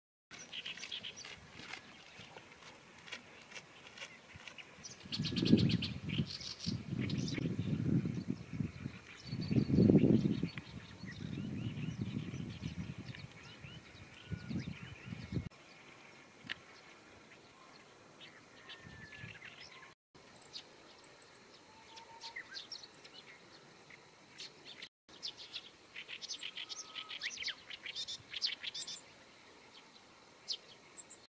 児島湖の朝2020年5月13日
朝の散歩の様子
ミサゴとホオジロ！